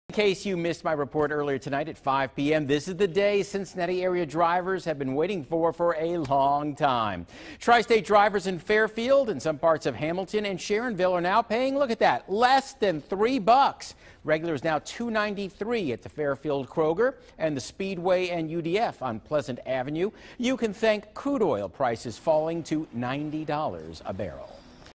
Cincinnati area gas prices fall below $3
John Matarese has the good news for Cincinnati area drivers.